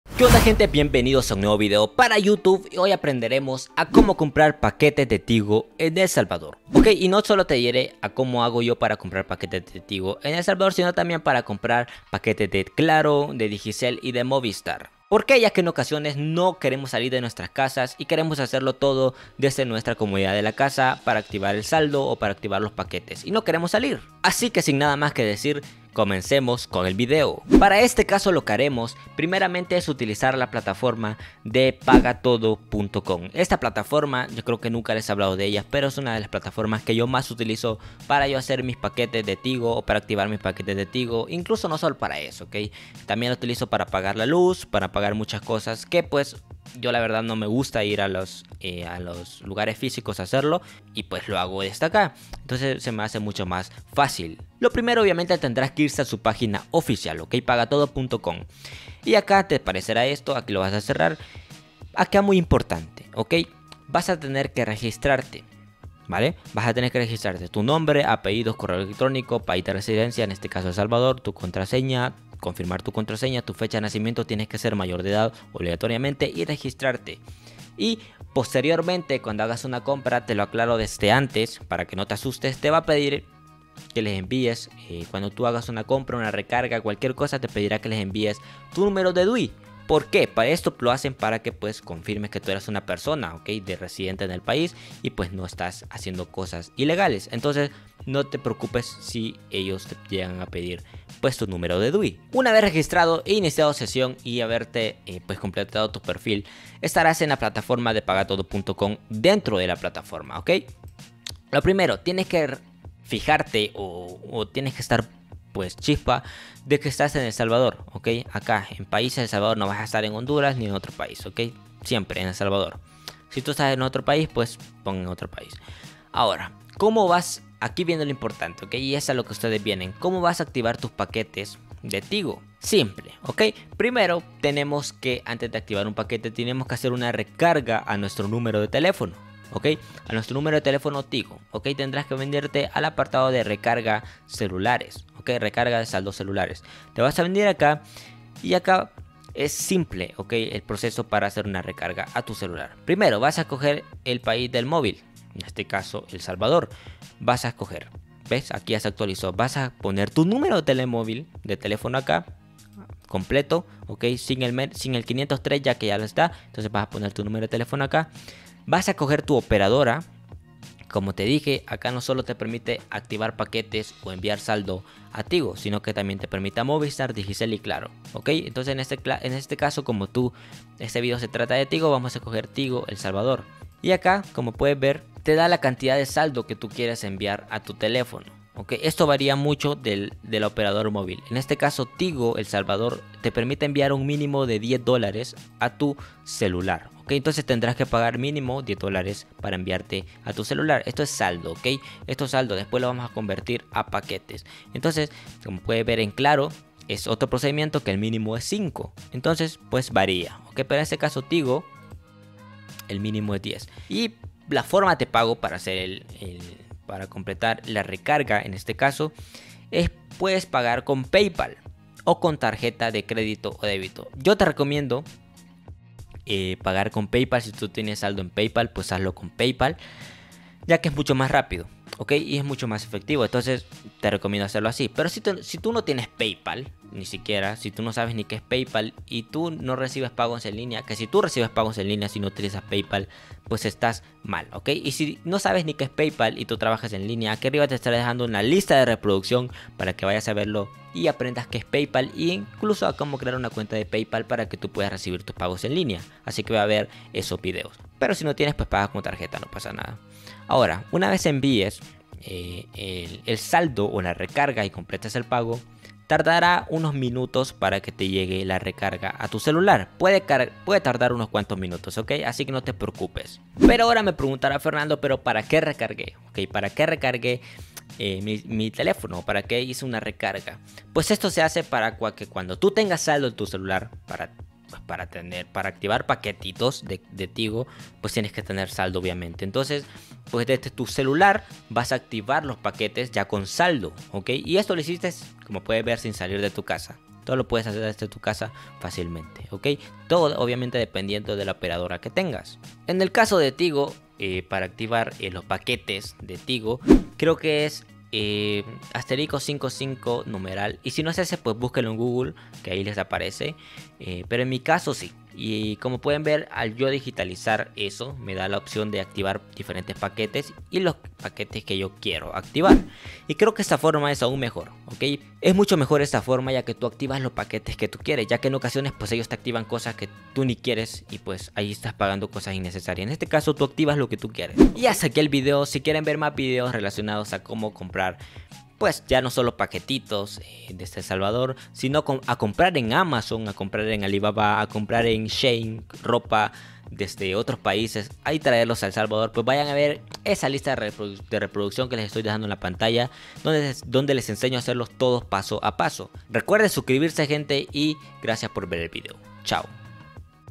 🛒 COMO COMPRAR PAQUETES EN TIGO EN EL SALVADOR (también para Claro, Digicel y Movistar) 🇸🇻 2023
Que onda gente, bienvenidos a un nuevo video, hoy aprenderemos a como comprar paquetes de tigo en El Salvador, te diré cómo hago yo, no solo para comprar paquetes de tigo, sino también para claro, digicel y movistar. Ya que en ocasiones pues estamos desde casa y necesitamos saldo o paquetes y no queremos salir.

Okey, para este caso, utilizaremos la plataforma de pagatodo, que para los que no la conozcan, es una empresa que te permite pagar recargas a celular, incluso pagar luz, cable e Internet, todo esto lo puedes hacer desde casa y sin ayuda de nadie, porque es muy fácil.